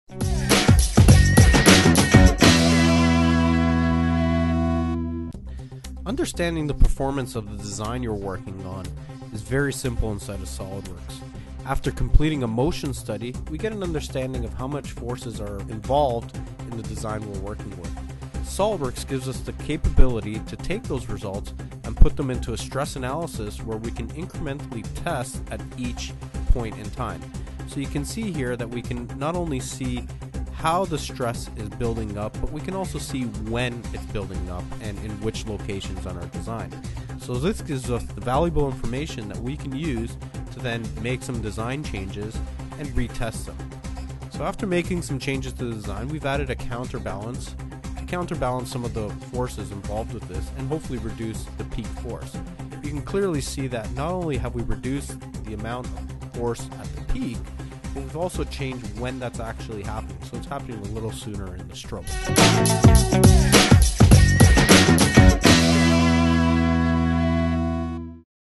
SolidWorks Simulation Stress Recovery
With SolidWorks Simulation added in, you can perform stress, factor of safety, or deformation analysis of components without setting up loads and boundary conditions. The required loads are obtained automatically from a calculated Motion Analysis study.